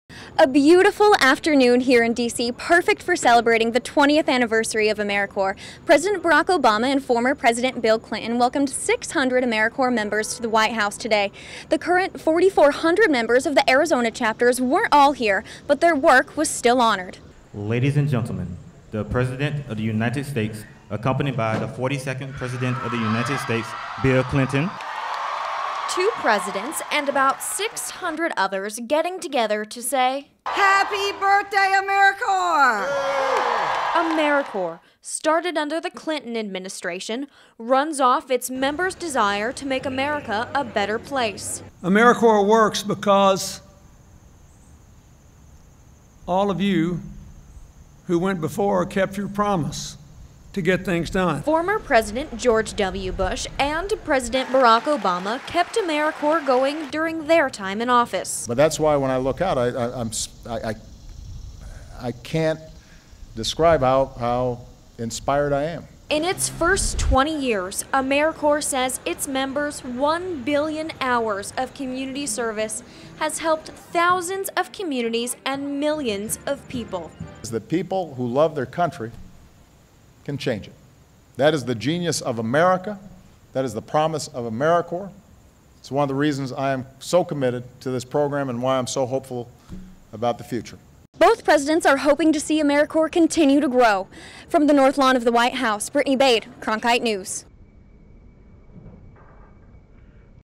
Obama, Clinton celebrate 20 years of AmeriCorps
AmeriCorps celebrates it's 20th birthday at the White House. Members and presidents were in attendance to honor the work of AmeriCorps whose mission is to make America a better place. Cronkite News reporter Brittany Bade takes us to the celebration.